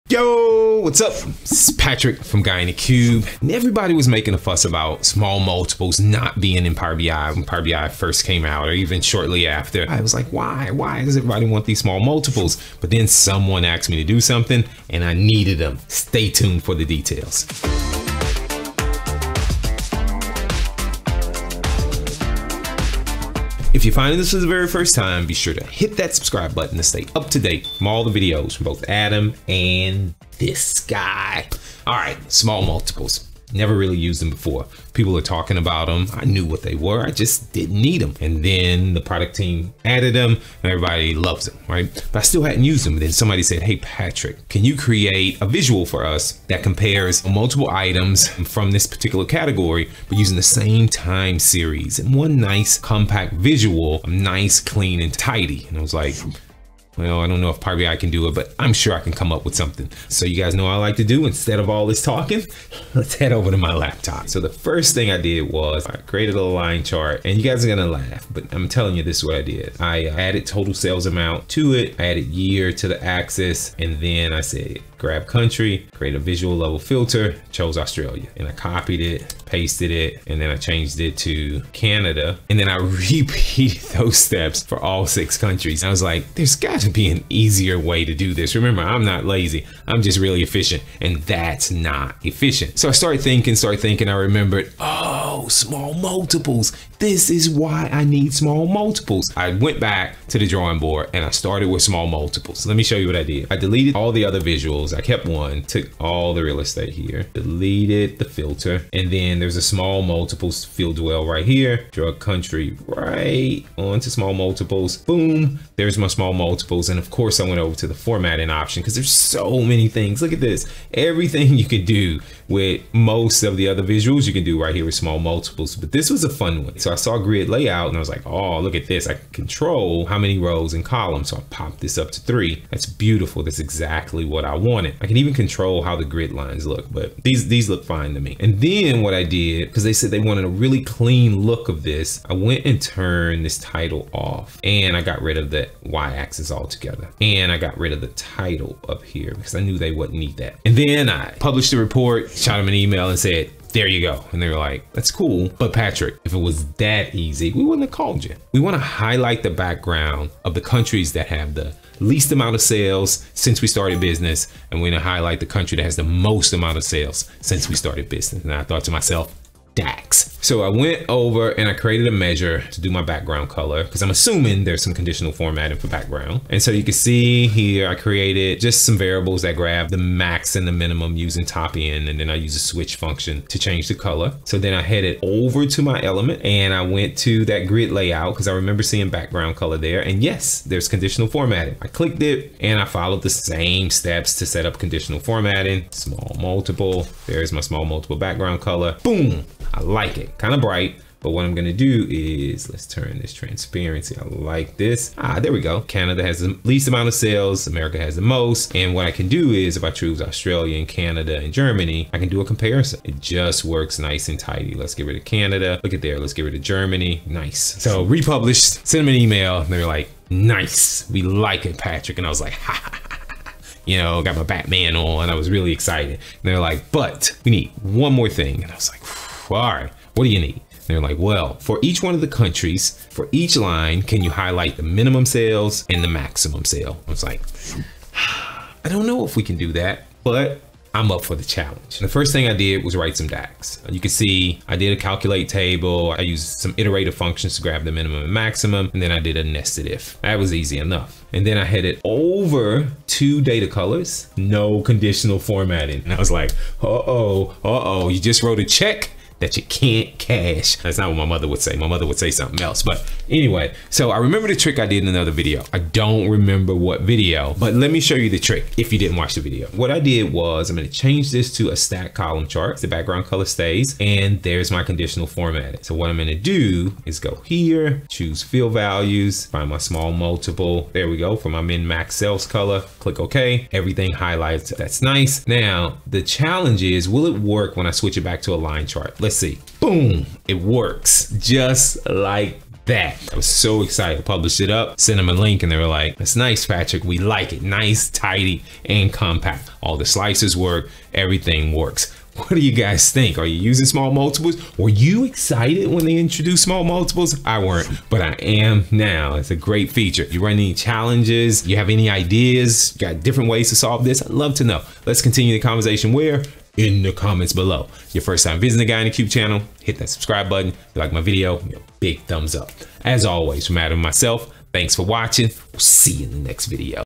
CRUSHING requirements with Small Multiples in Power BI
Check out how using Small Multiples in Power BI can help you CRUSH requirements and satisfy business requests to make amazing reports! Patrick also has a neat trick for you.